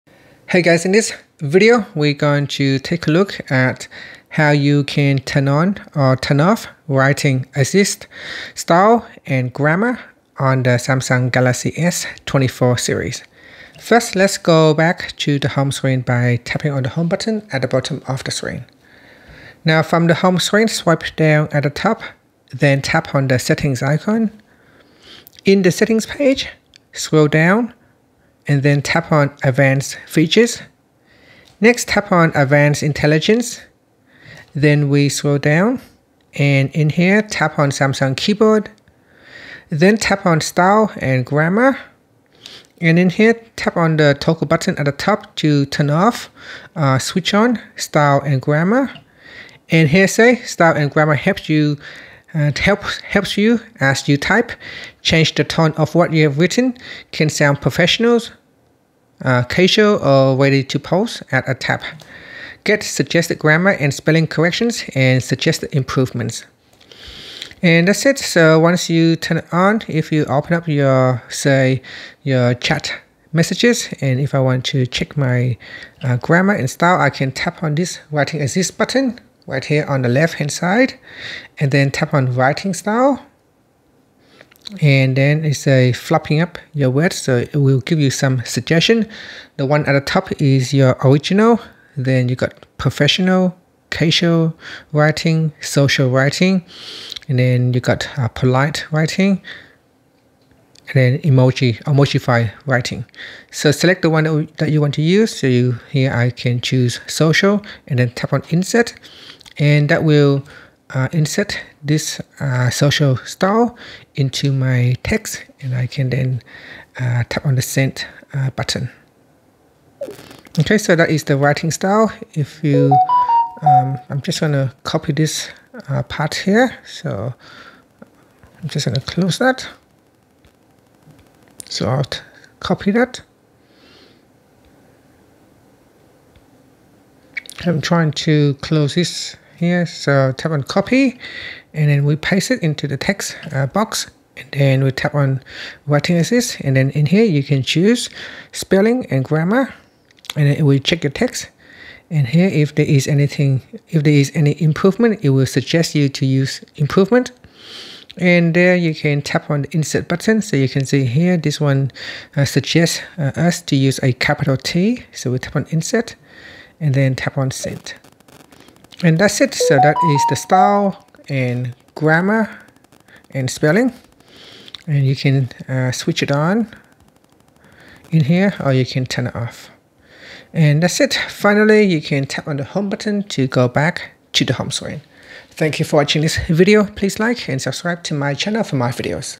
Galaxy S24/S24+/Ultra: How to Turn On/Off Writing Assist Style & Grammar
Maximize your Samsung Galaxy S24 experience with our step-by-step guide on enabling and disabling Writing Assist Style & Grammar. Whether you're a grammar enthusiast or just looking to enhance your text composition, this tutorial has you covered! Learn the ins and outs of customizing your writing experience on the Samsung Galaxy S24, S24+, and Ultra running Android 14. From enabling helpful suggestions to turning off intrusive corrections, we've got the tips you need. Watch now and elevate your writing game with ease.